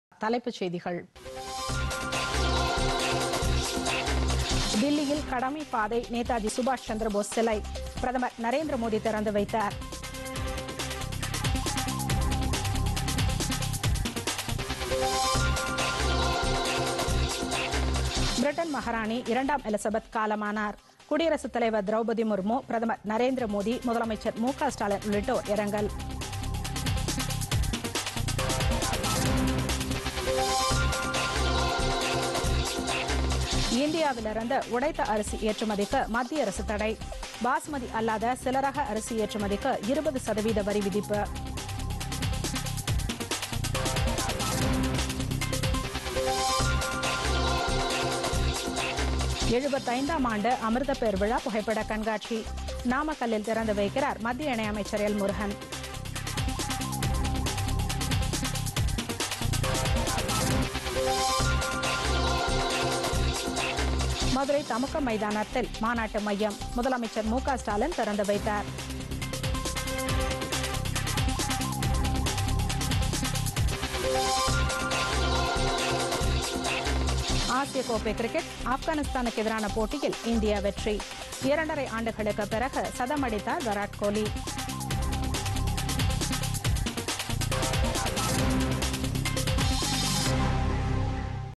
பொதிகை காலை 11.00 மணி தலைப்புச் செய்திகள் [09.09.2022]PodhigaiTamilNews
1) தில்லியில்  கடமை பாதை , நேதாஜி சுபாஷ்சந்திரபோஸ் சிலை -  பிரதமர் நரேந்திர மோதி திறந்து வைத்தார்.

2) பிரிட்டன் மகாராணி 2ம் எலிசபெத் காலமானார் - குடியரசுத் தலைவர் திரவுபதி முர்மு,  பிரதமர் நரேந்திர மோதி , முதலமைச்சர் மு க ஸ்டாலின் உள்ளிட்டோர் இரங்கல்.

3) இந்தியாவிலிருந்து உடைத்த அரிசி ஏற்றுமதிக்கு மத்திய அரசு தடை - பாசுமதி அல்லாத சில ரக அரிசி ஏற்றுமதிக்கு 20 சதவீத வரிவிதிப்பு.

4)  75 ஆம் ஆண்டு  அமிர்த பெருவிழா புகைப்பட கண்காட்சி - நாமக்கல்லில் திறந்து வைக்கிறார் மத்திய  இணையமைச்சர் எல் முருகன்.

5) மதுரை தமுக்கம் மைதானத்தில் மாநாட்டு மையம் - முதலமைச்சர் மு க ஸ்டாலின் திறந்து வைத்தார்.

6) ஆசிய கோப்பை கிரிக்கெட் -  ஆப்கானிஸ்தானுக்கு எதிரான போட்டியில் இந்தியா வெற்றி.  இரண்டரை ஆண்டுகளுக்கு பிறகு சதம் அடித்தார் விராட் கோலி.